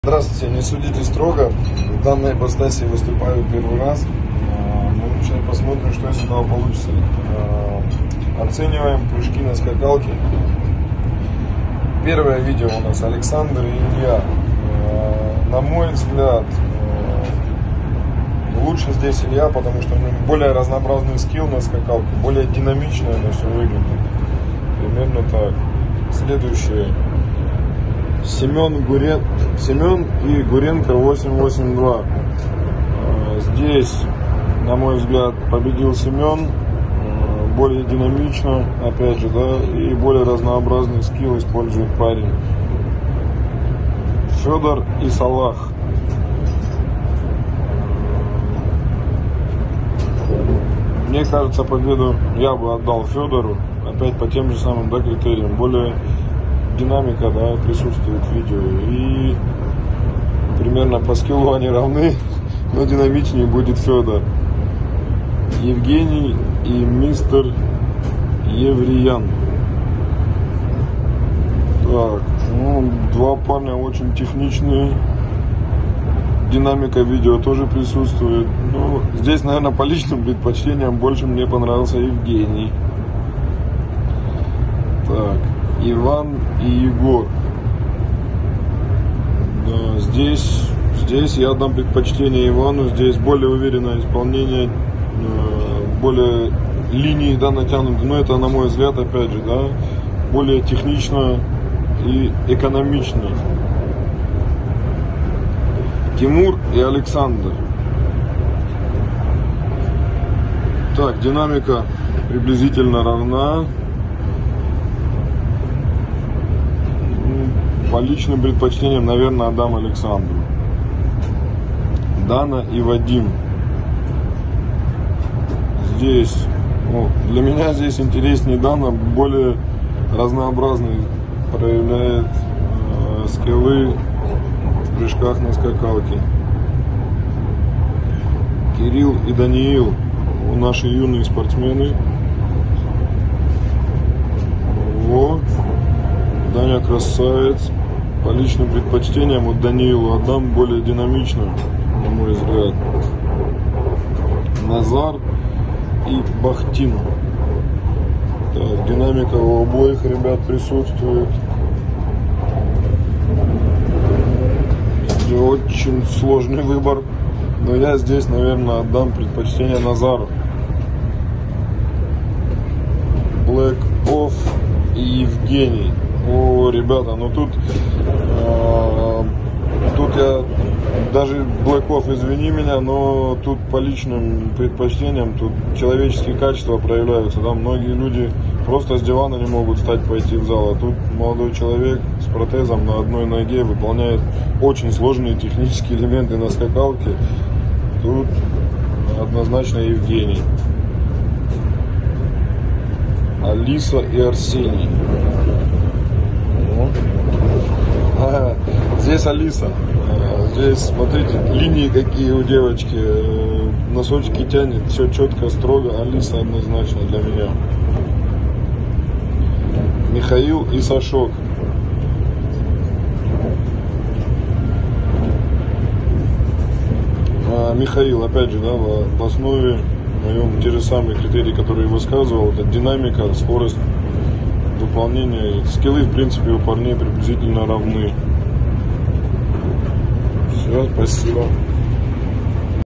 этап ВебКубок БоксаСпб3 | подведение итогов | Судья Денис Левизов
Подводит итоги II этапа ВебКубокБоксаСпб3 тренер сборной Санкт-Петербурга по функциональной подготовке Денис Левизов